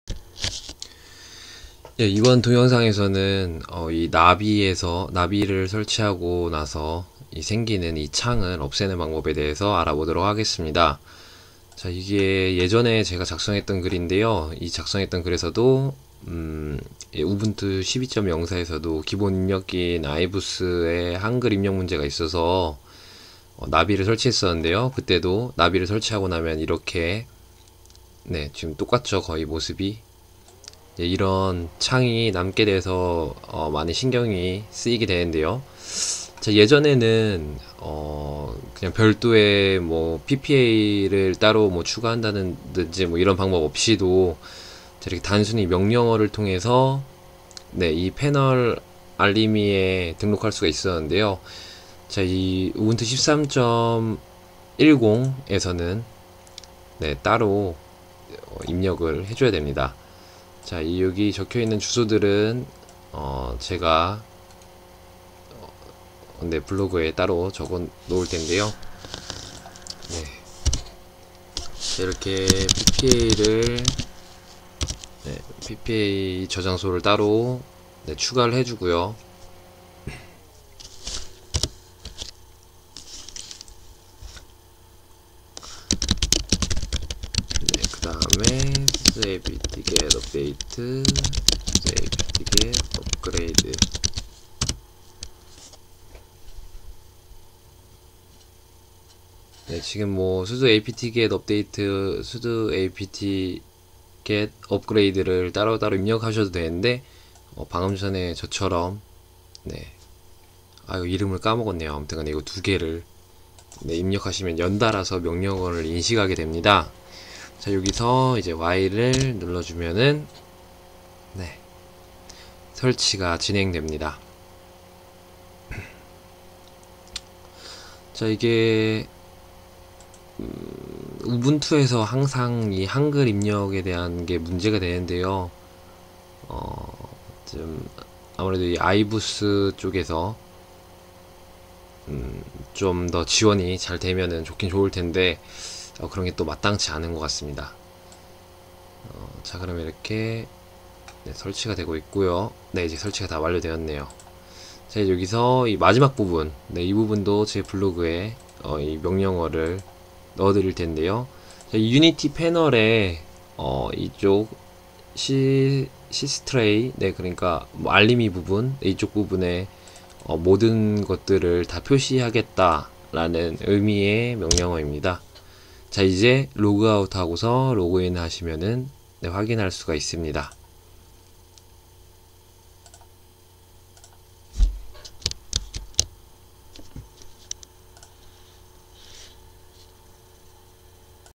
우분투 13.10 한글입력 문제(2) - 나비입력기 패널에 추가하기
우분투 13.10의 한글입력 문제로 인한 나비입력기 설치 중 나비입력기를 패널에 추가하는 동영상입니다.
자세한 설명은 에서 확인할 수 있습니다.